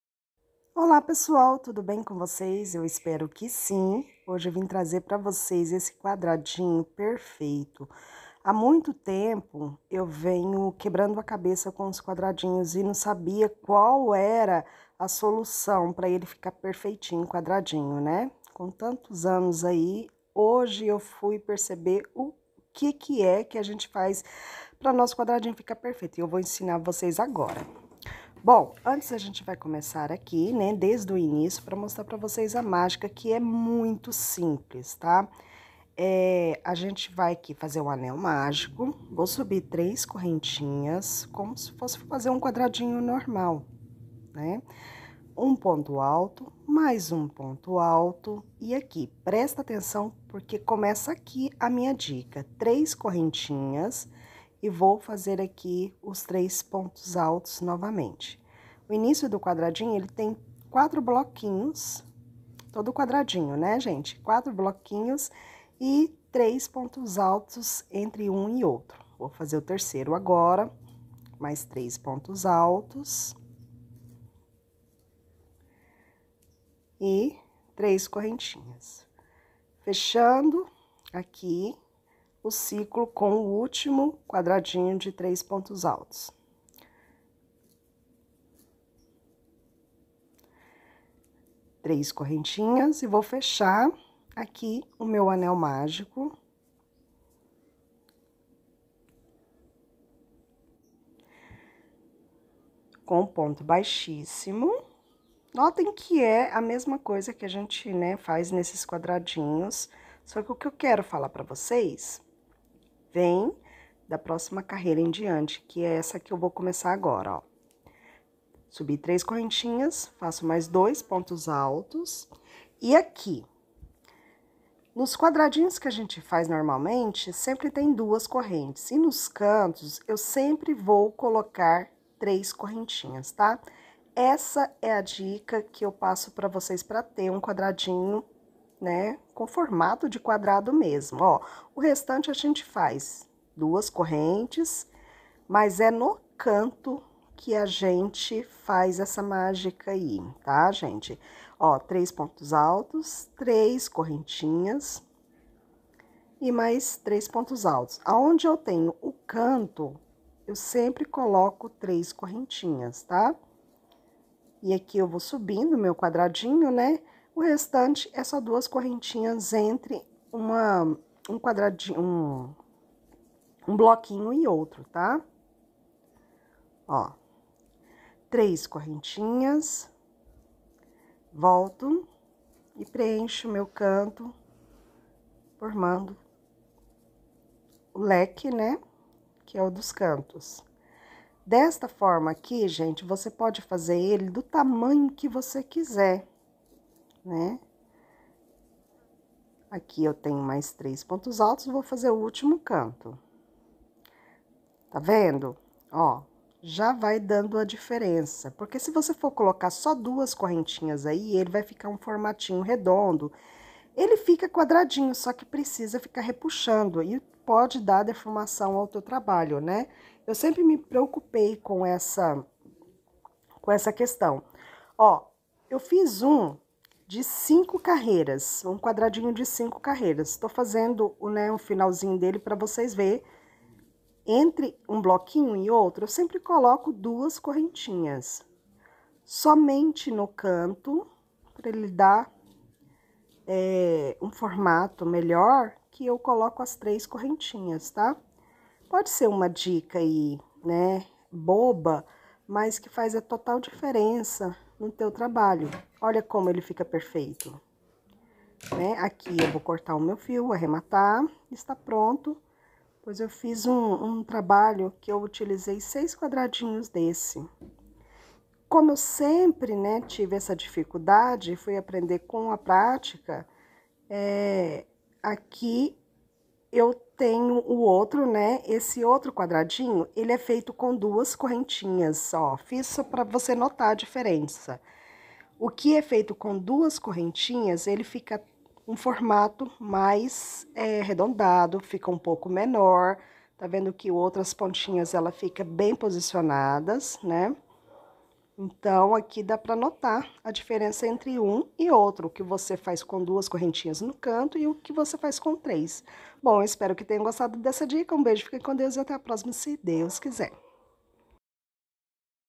quadradinho de crochê perfeito
aprenda de uma forma fácil a fazer um quadradinho de crochê perfeito

Whatssap Melo Veríssimo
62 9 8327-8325

Apoio circulo 🥰